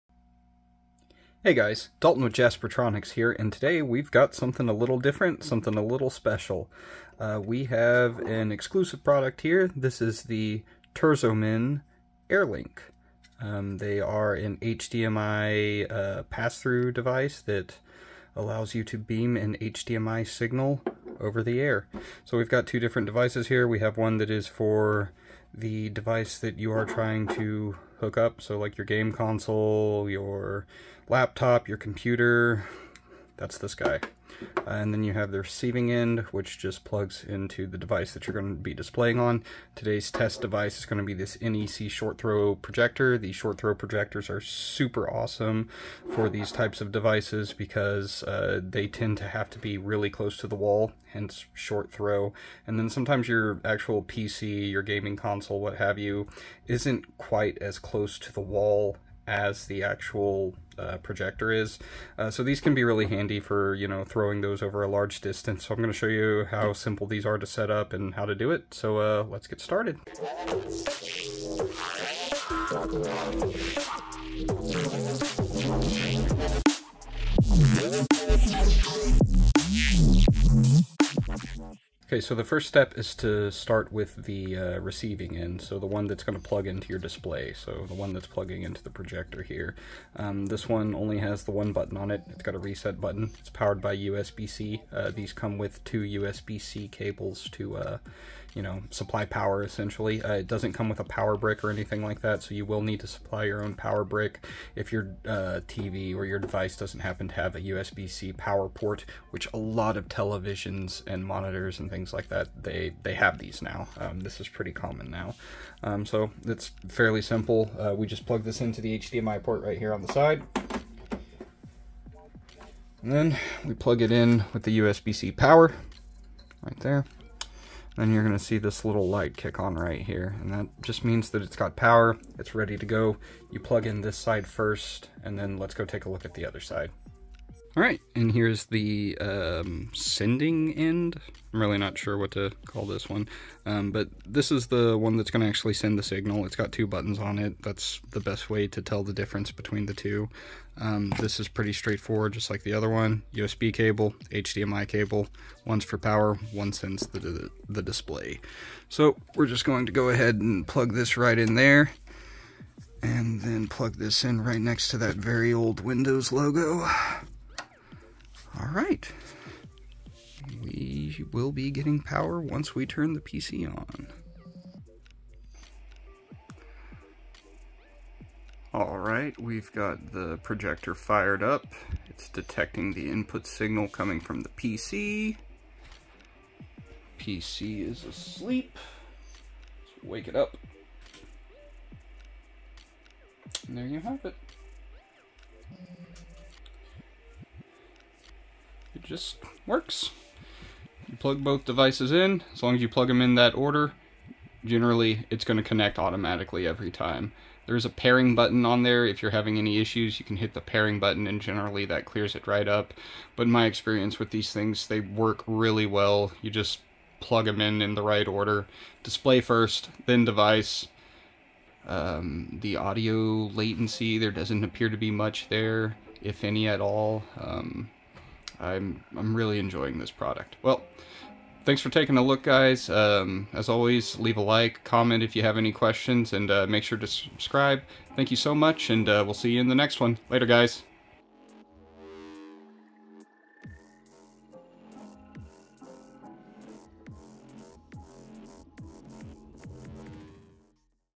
Terzomen AirLink HDMI Display Tutorial
Hey guys, today we will be looking at something pretty unique and different to the channel. Today we are looking at the Terzomen AirLink display adapter for HDMI passthrough. This light weight and easy to use plug and play adapter can support full 1080p and seamless audio transmission over the air.

Support independent music! Music by INDOL